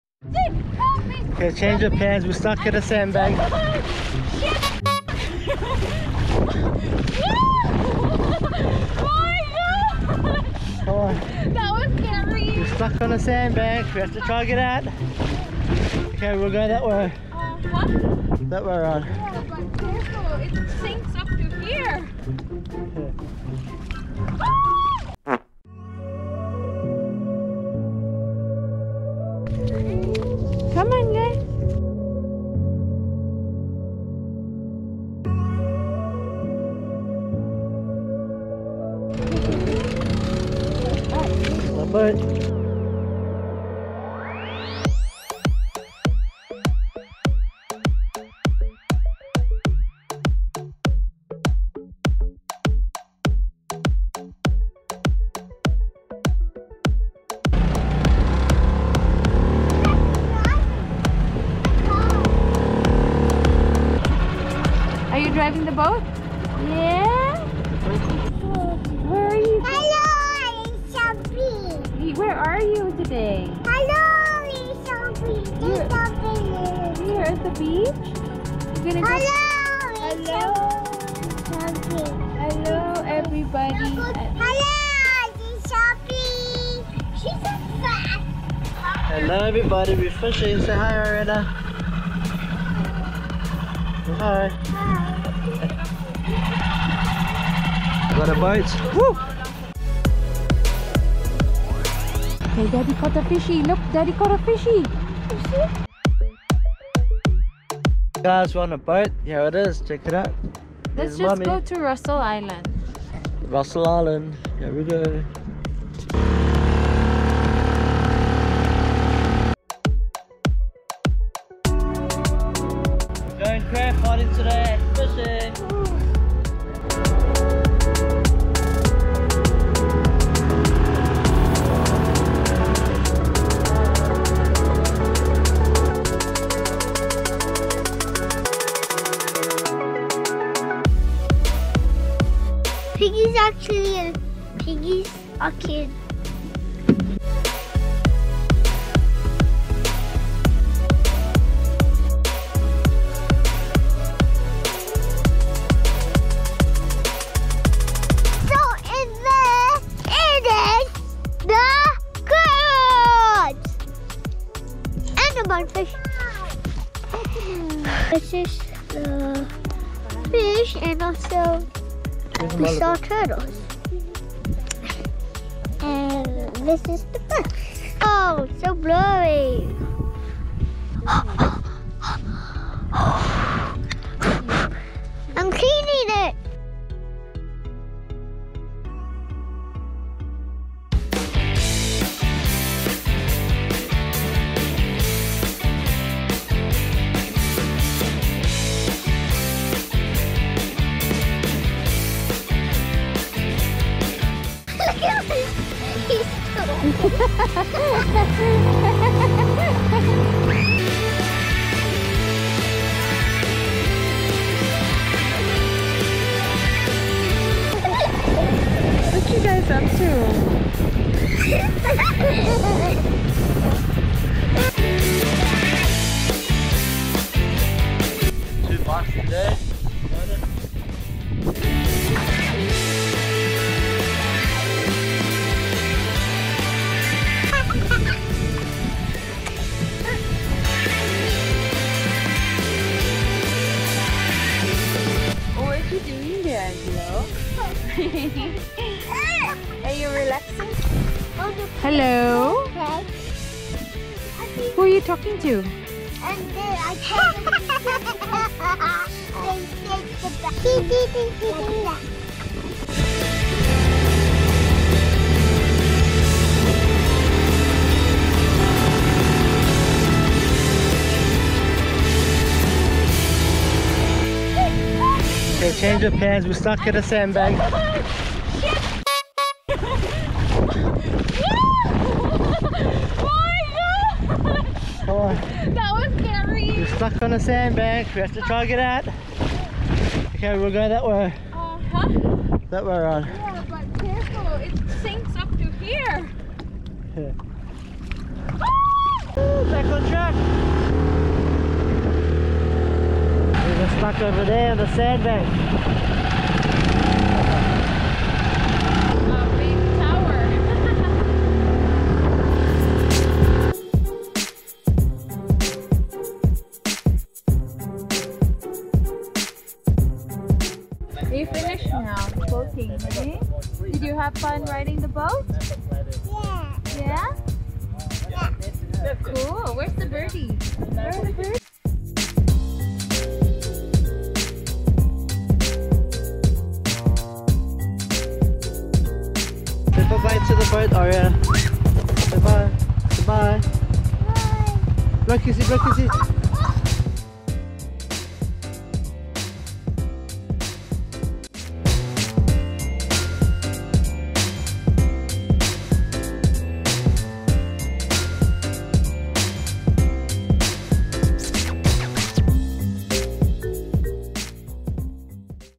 Nearly Spent Night Stuck on a Sandbank | Cabbage Tree Point | ep 9
Nearly Spent Night Stuck on a Sandbank | Cabbage Tree Point

We Hired a boat and got stuck on a massive sandbank. The boat had a Broken Depth Finder grrrrrr

We are a family who love to explore and go on great adventures. Come with us while we Camp, Boat, Fish and explore the world.

PEACE!!